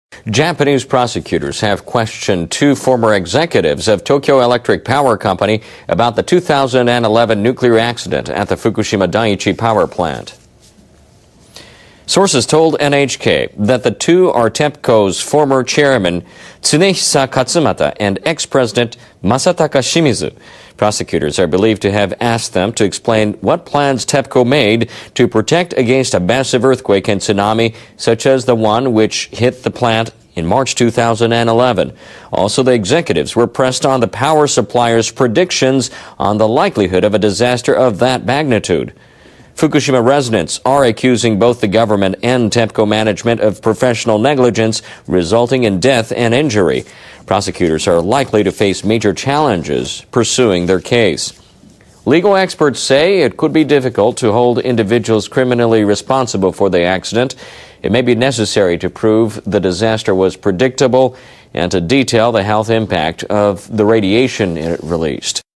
Prosecutors question former TEPCO executives
Japanese prosecutors have questioned 2 former executives of Tokyo Electric Power Company in connection with the 2011 nuclear accident at the Fukushima Daiichi power plant.

Sources have told NHK that the 2 are TEPCO's former chairman Tsunehisa Katsumata and former president Masataka Shimizu.

The prosecutors are believed to have asked them to explain what kind of measures their company had taken against tsunami risk and how likely they had predicted such an accident would be.

Fukushima residents are accusing the government and TEPCO officials at the time of the accident of professional negligence resulting in death and injury.

Some legal experts say it may be difficult to hold individuals criminally responsible for the accident.

They say that's because the prosecutors need to prove the disaster was predictable. The prosecutors also need evidence to prove the health impact of radiation from the Fukushima Daiichi plant.

The prosecutors are believed to be proceeding cautiously with their investigation into the case.

Jan. 24, 2013 - Updated 09:06 UTC (18:06 JST)